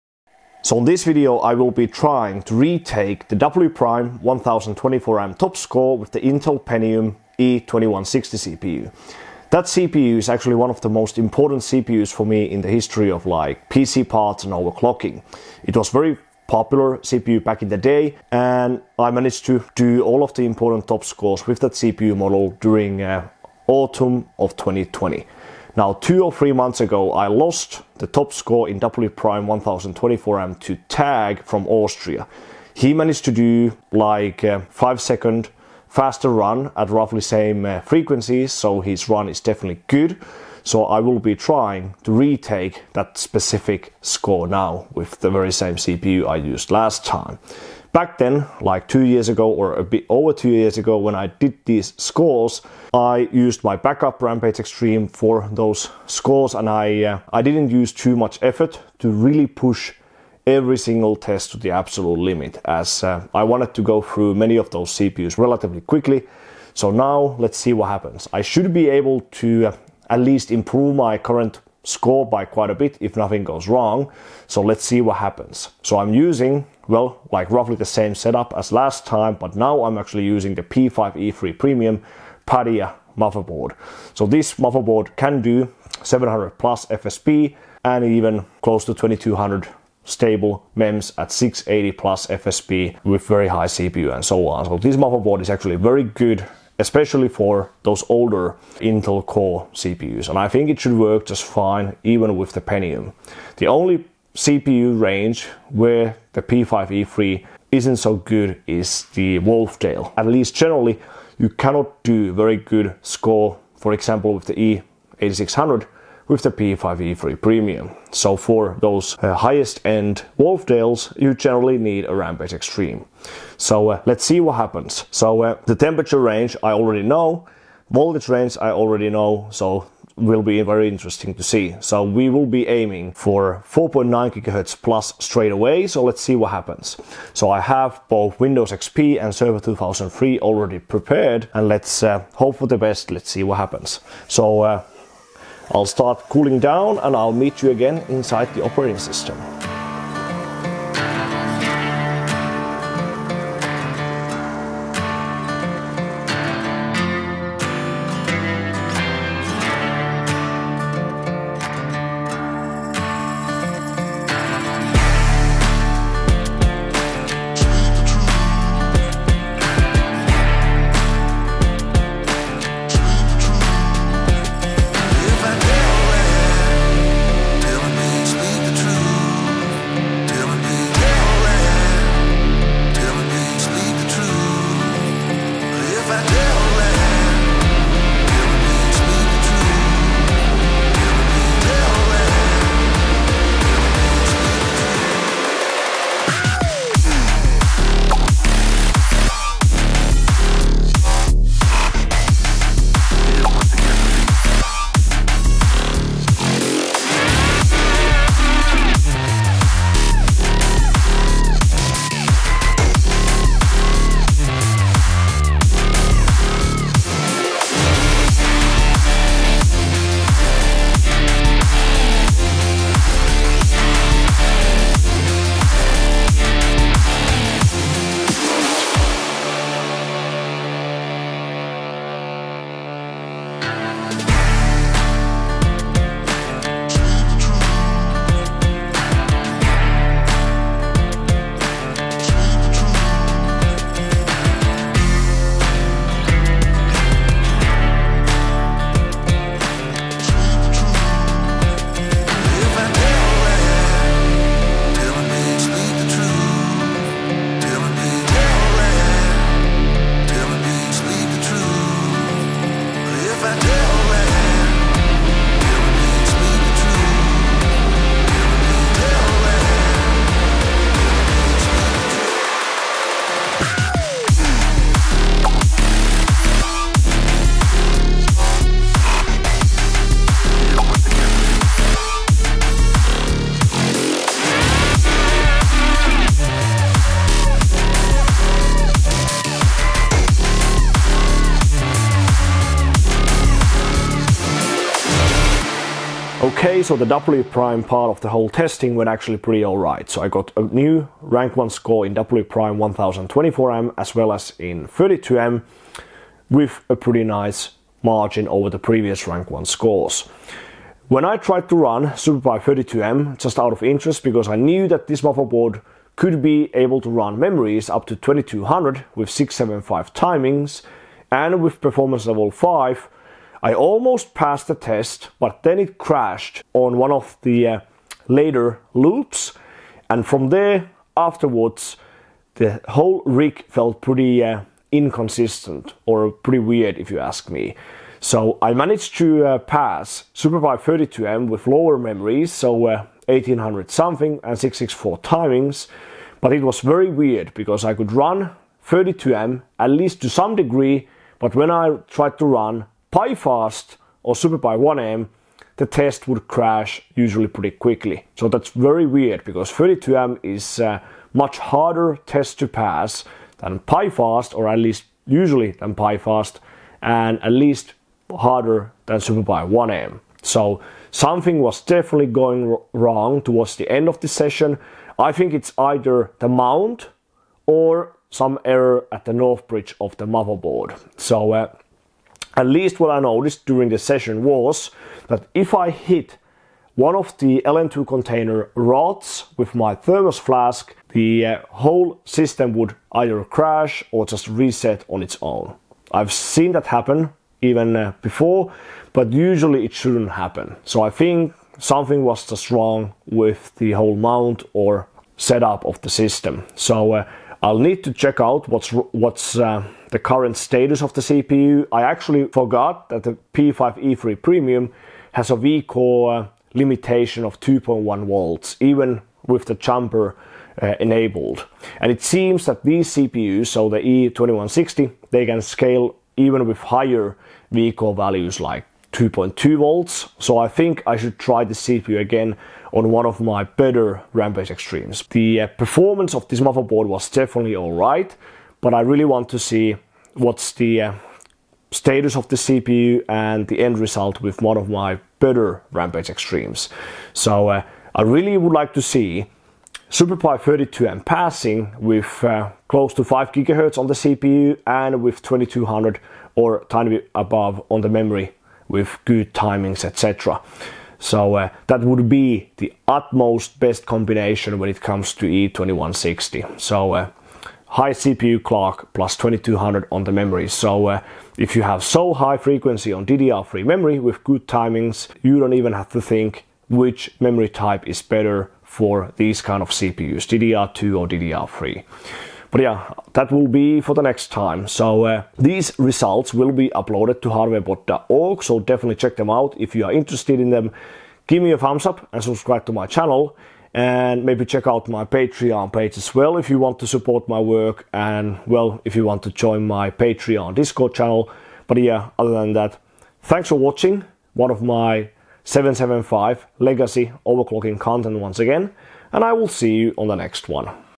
Taking Back What I Deserve - Intel E2160 Smashes Records on ASUS P5E3 Premium
As I lost WPrime 1024M top score with the Intel Pentium E2160 around 2 or 3 months ago, I wanted to take it back quickly during this session as E2160 is one of my favourite LGA 775 CPUs that ever existed. Last time when I ran this CPU on LN2 I used my backup Rampage Extreme and I didn't really push every single test to the absolute limit.

This time I decided to go with the P5E3 Premium after seeing the excellent performance I had with the E6750 CPU and the result went pretty good actually, at least for a moment. I managed to take the Rank #1 score back quite easily in WPrime 1024M, with high margin over the previous top score by Tagg, and a nice improvement in WPrime 32M as well. Super PI 32M I started getting issues as the whole rig went very unstable, or very incosistent. Something was definitely wrong with the mount as I had crashes and resets if I hit one of the LN2 pot rods with my thermos flask.

Luckily I got one Super PI 32M run through with quite decent memories at 18xx with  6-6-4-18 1T timings, but I was very close on passing the test even with DDR3-2200+ memories. I will have to rerun the test with one of my best Rampage Extremes later.